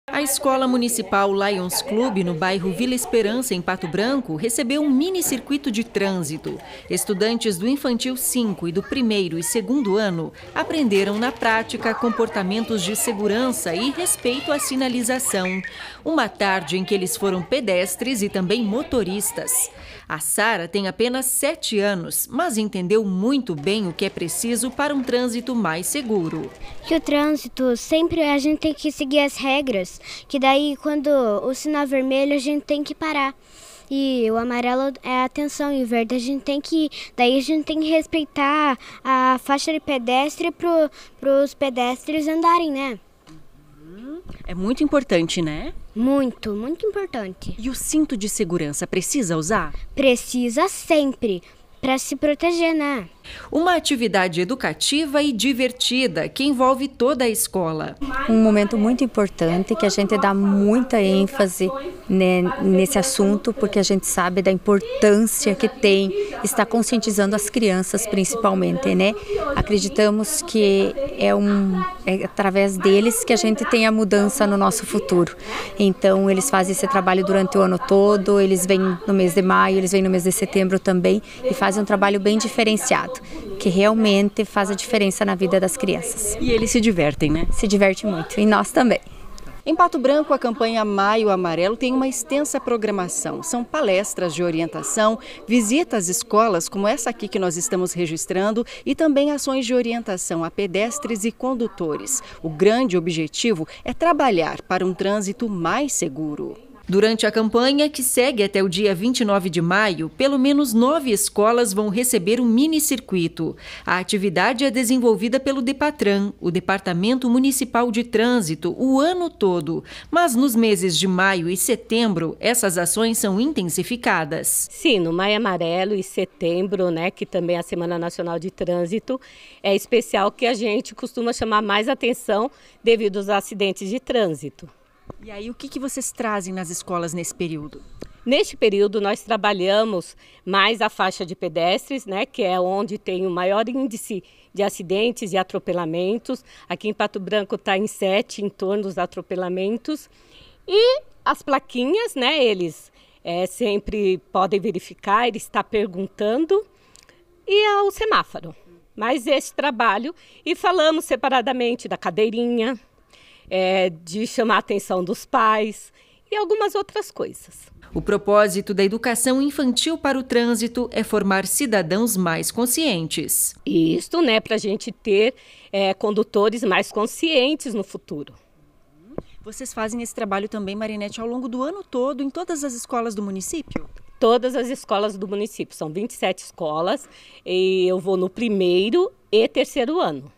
Mini circuito de trânsito faz parte das ações da Campanha Maio Amarelo
Trânsito: Na programação da Campanha Maio Amarelo, várias ações são realizadas em Pato Branco. Estudantes da rede municipal aprendem com agentes do DEPATRAN, na prática, a respeitar as leis de trânsito.